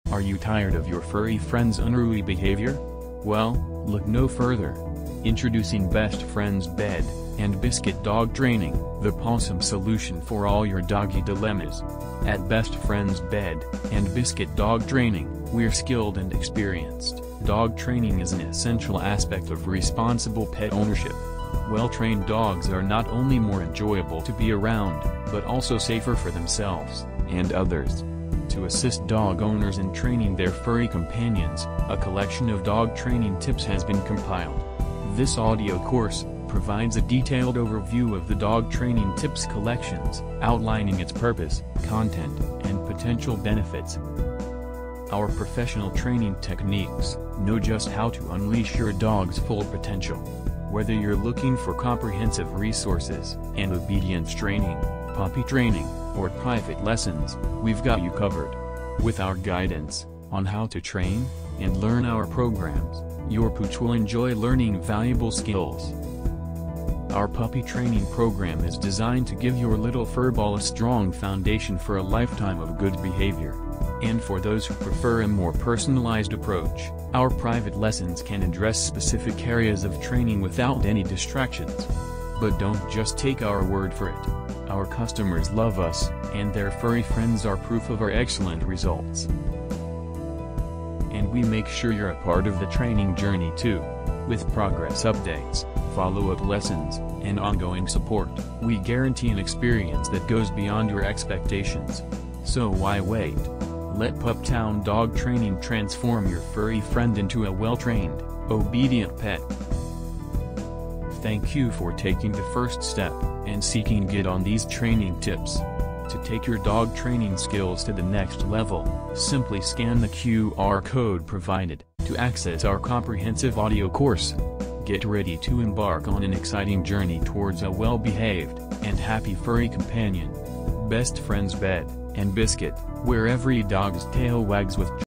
"Paws & Play: A Guide to Happy, Healthy Dog Ownership"
Start 2024 on the right paw by mastering leash training. Learn how to enjoy tangle-free walks with your furry friend. Discover expert tips and tricks to make your daily outings enjoyable for both you and your pup.
Unlock the secrets of positive reinforcement in dog training. We dive deep into the science behind why this approach works wonders. Find out how you can use treats, praise, and other rewards to transform your dog's behavior.